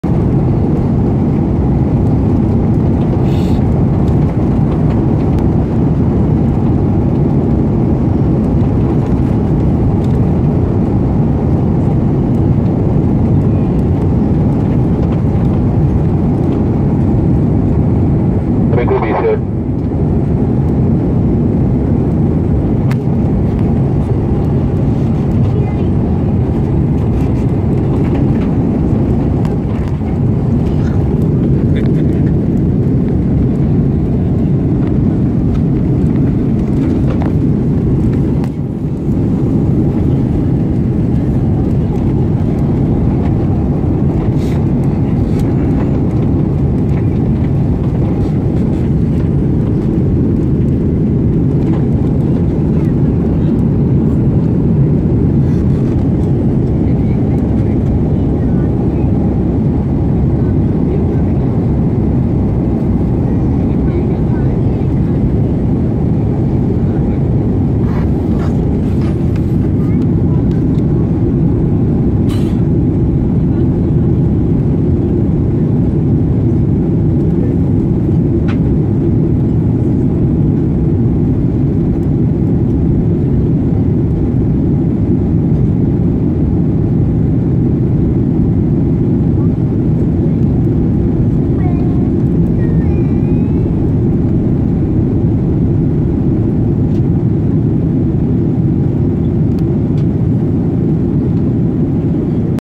Sri Lanka air line experience terrible flight
Flight from Colombo to lahore